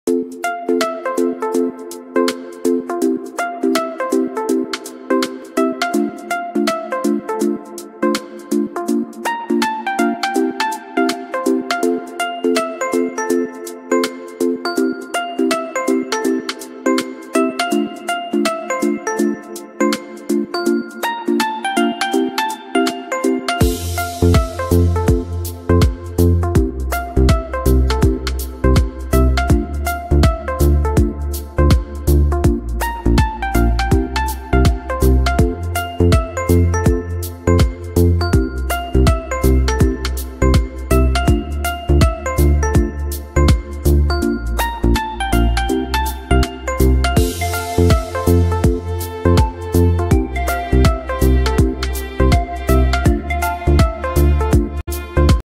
try these 2 refreshing tea recipes!!! good for immunity 😋ಟ್ರೈ ಮಾಡಿ ಬಾಯಿ ಚಪ್ಪರಿಸುವ 2 ಚಹಾ
Musician: @iksonmusic